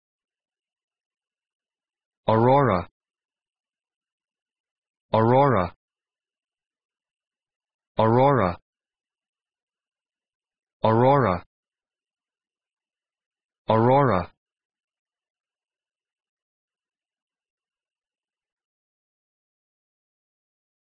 How to pronounce aurora
aurora American English pronunciation. How to pronounce aurora correctly. How to say aurora in proper American English.